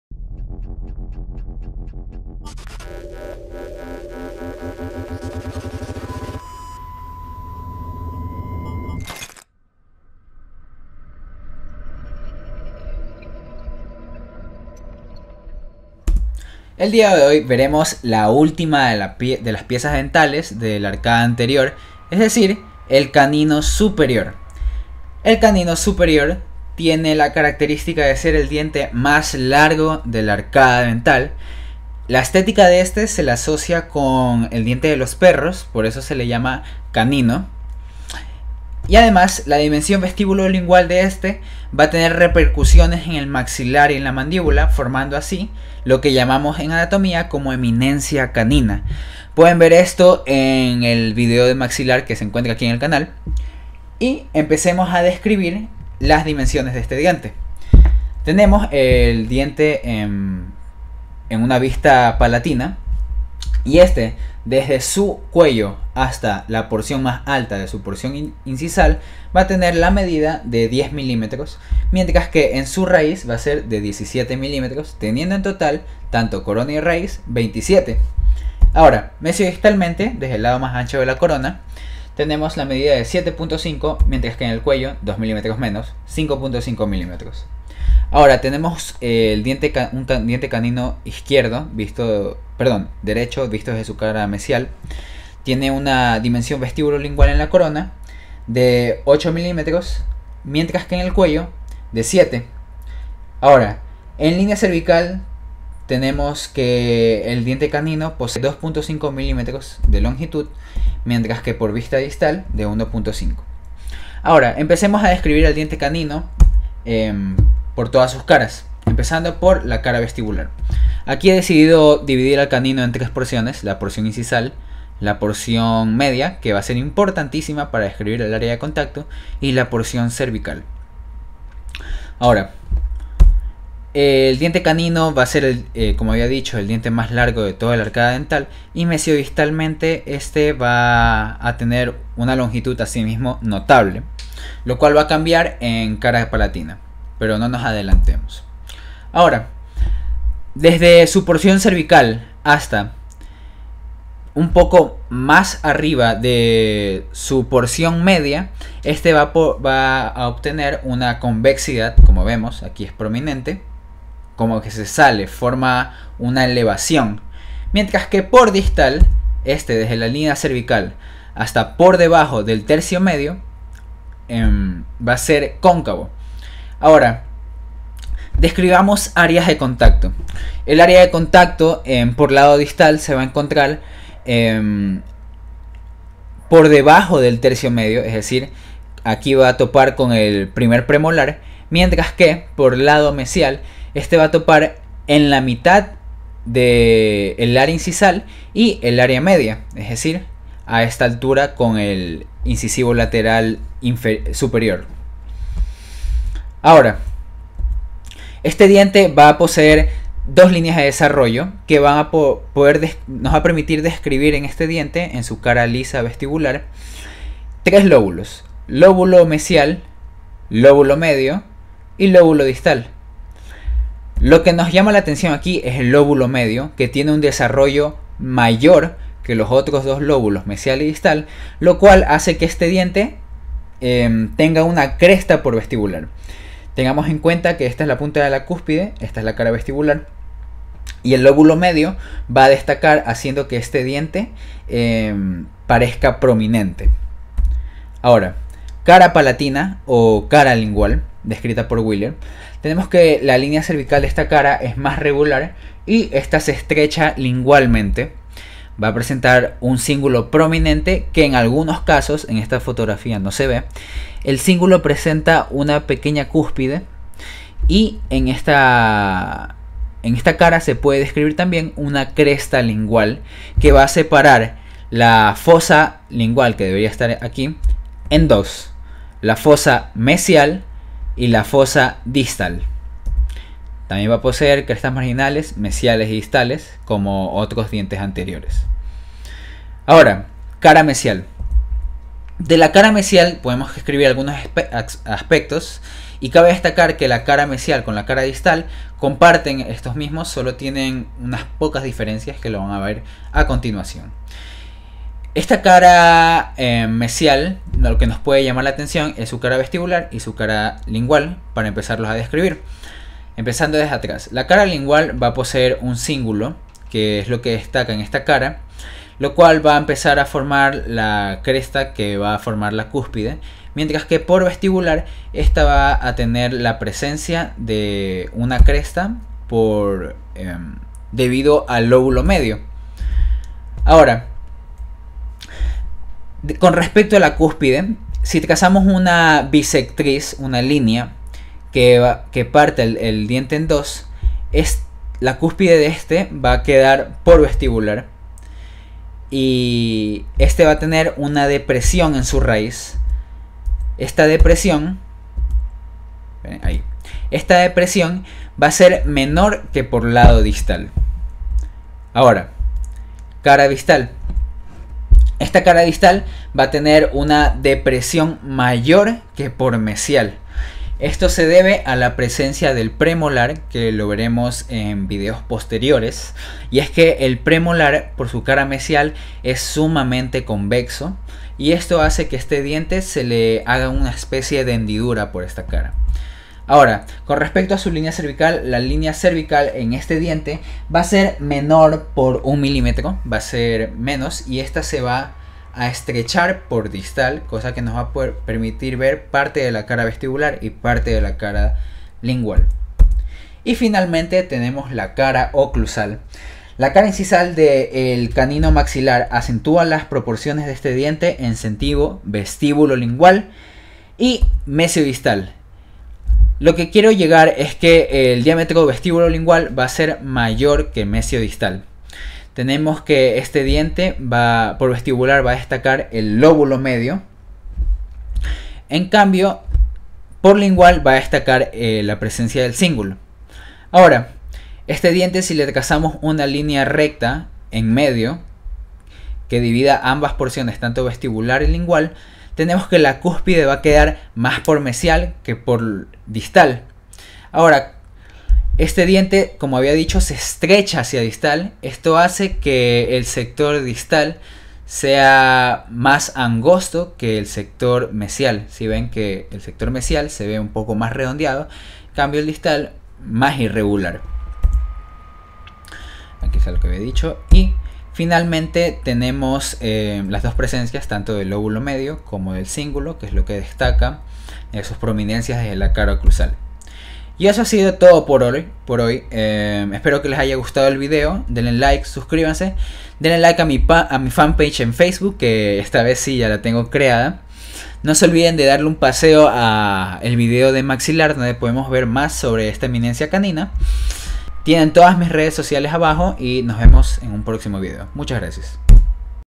Anatomía Dental - Canino Maxilar (Dimensiones, Caras, Raíz)
Estudiante de Odontología de la Universidad Católica Santiago de Guayaquil.